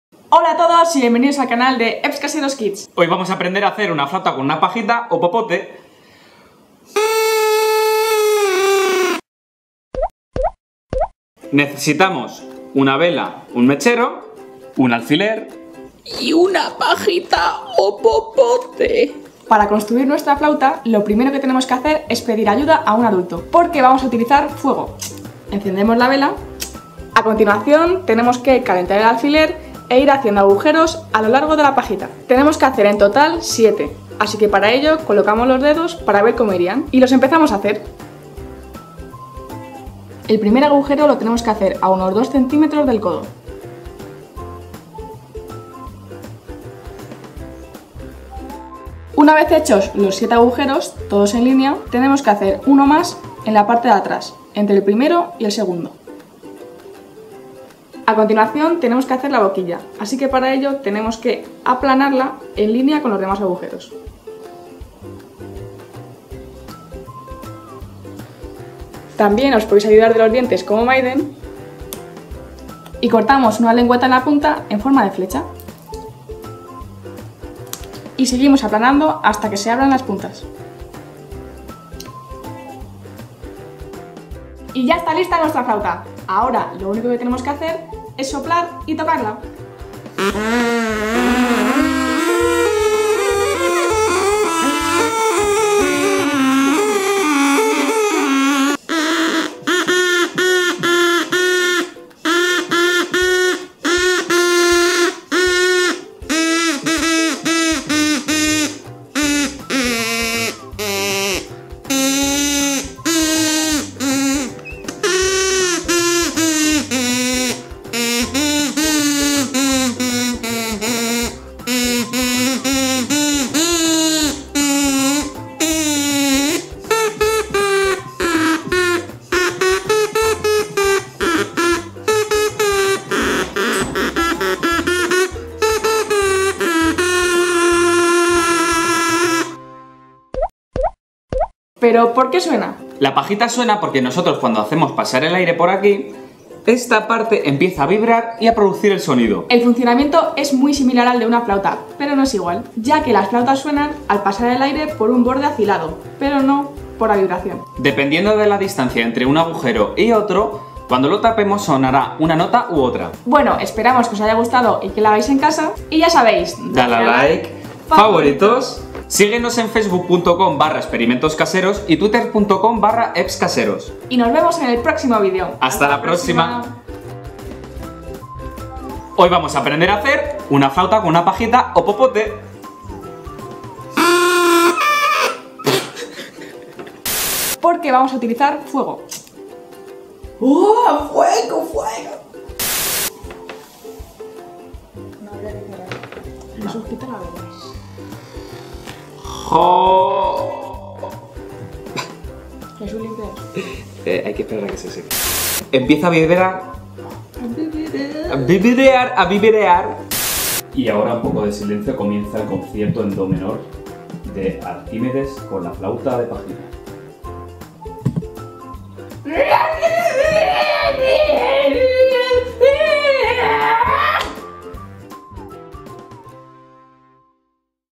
Flauta casera con una pajita, cañita o popote (Experimentos Caseros)
Aprende a hacer una divertida flauta en casa con una simple pajita, un alfiler y unas tijeras. ¿Adivinarás las canciones que tocamos en el vídeo? ;)

Envíanos tus cartas y dibujos a:
ExpCaseros
Apartado Postal nº 38088
Madrid, España